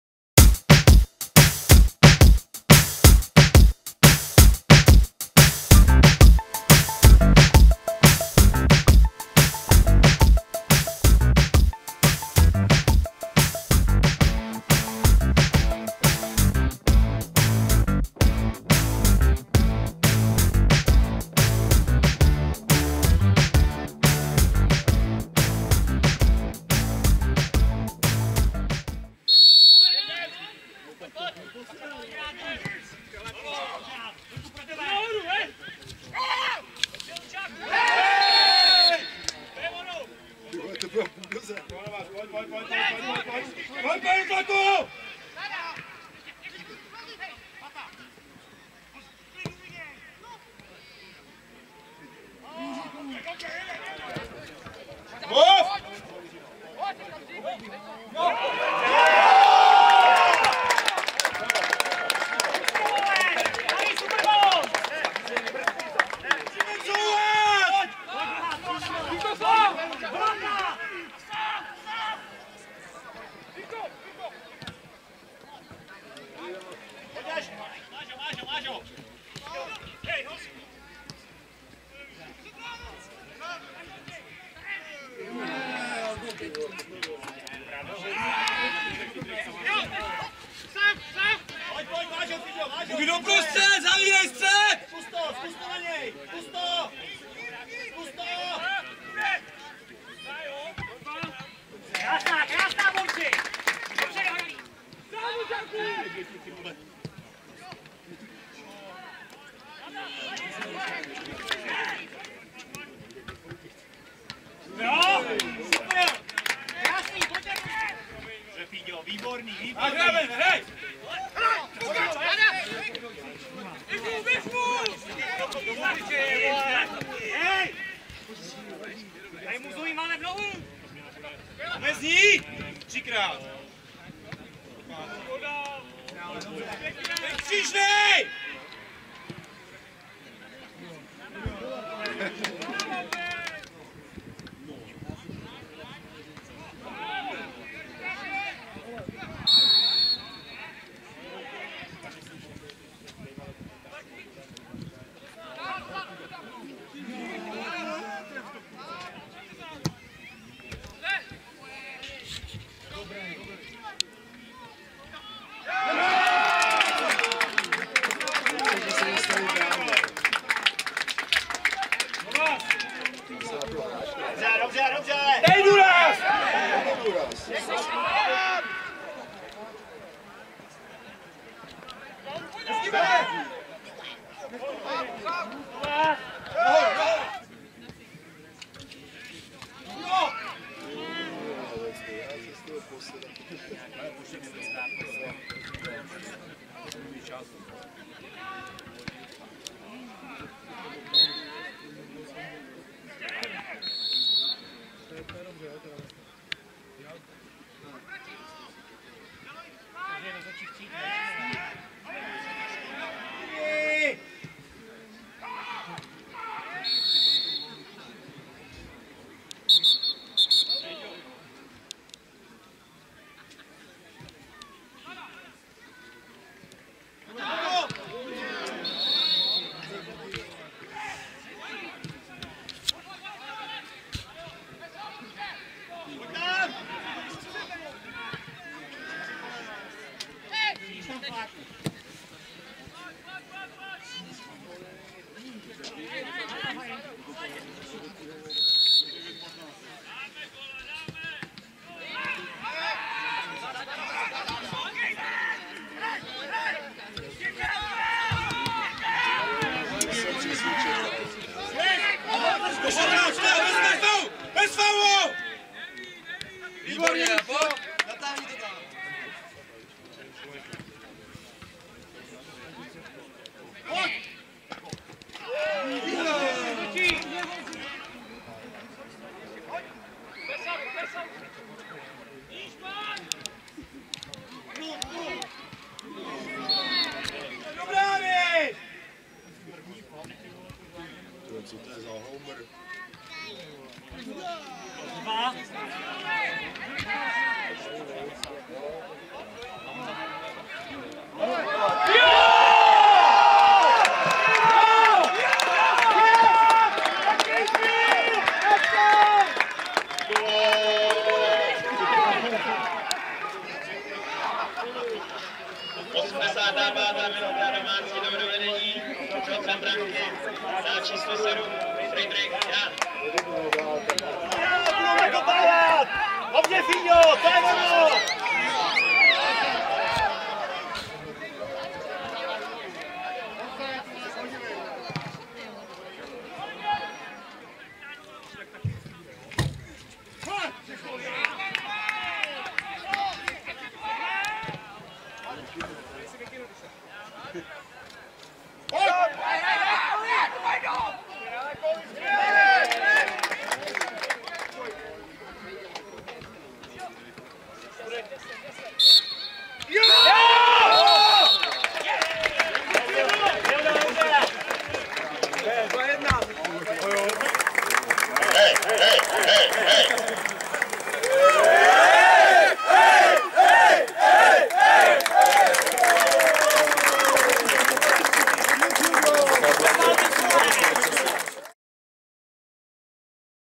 Sestřih Třebeš B - FC Nový HK 2:1 (1:1)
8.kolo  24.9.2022
AM GNOL 1. A třída mužů
Třebeš B - FC Nový HK  2:1 (1:1)